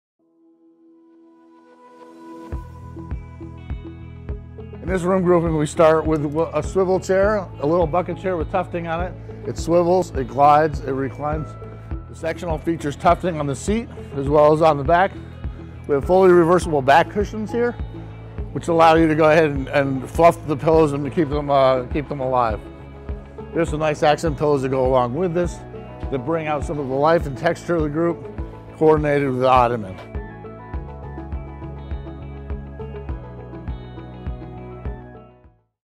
Ashley Furniture Homestore India | 25003 | Flintshire Collection
Master the art of mid-century inspired design with the Flintshire 2-piece sectional in an earthy auburn upholstery with menswear texture. Sporting all the signature elements you love, including crisp track arms, tapered feet, box cushions and button tufting, what a retro-chic look that feels right on trend. Reversible back cushions allow you to flip and fluff with ease.